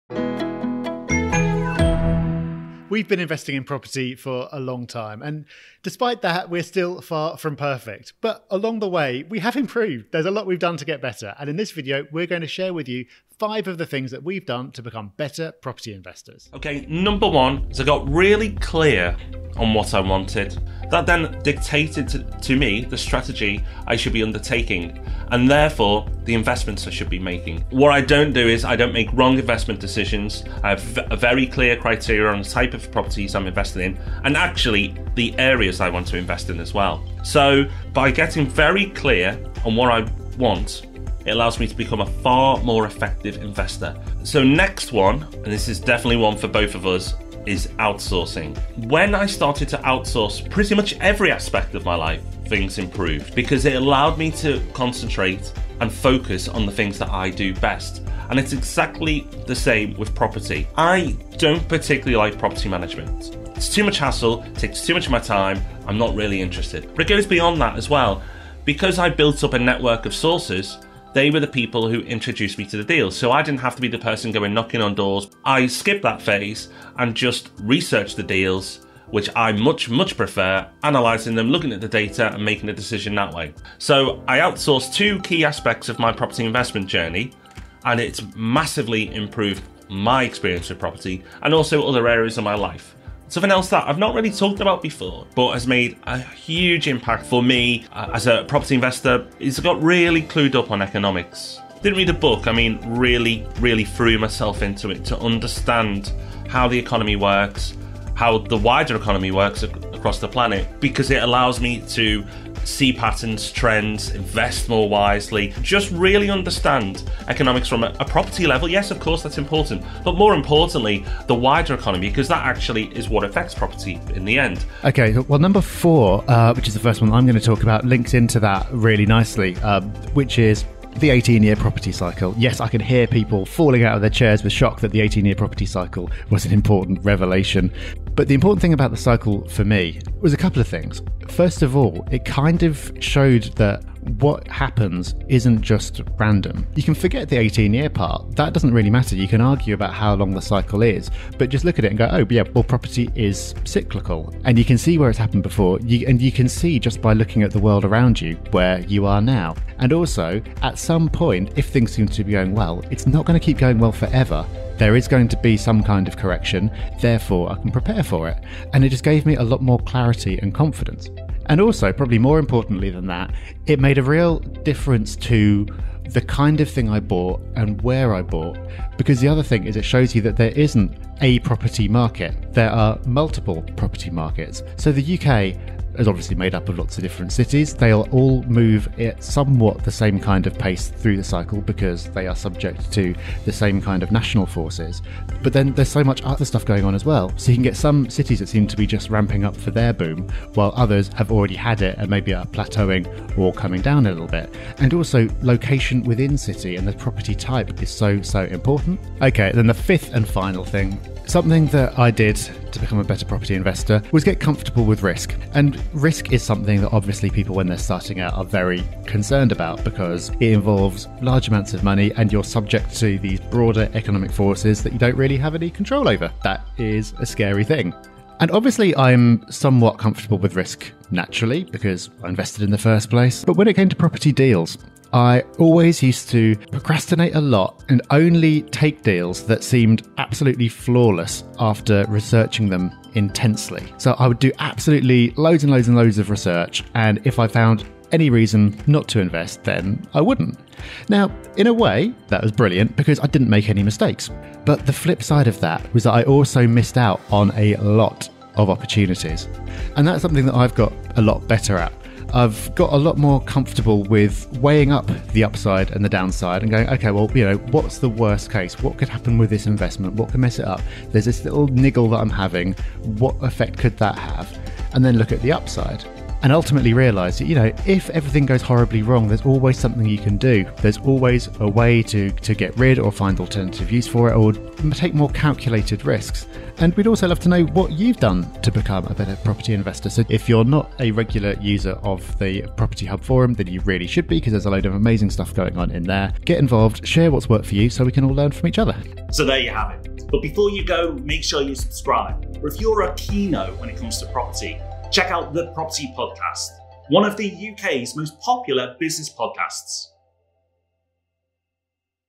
Top 5 ways to become a better property investor! | Property Hub
We never claim to be the greatest investors in the world, but one thing’s for sure – we’re a lot better than we used to be.

But what exactly did we do to become better property investors?

More importantly, what can you do?

Hit play and jump into this week’s video and learn 5 ways you can become a better property investor!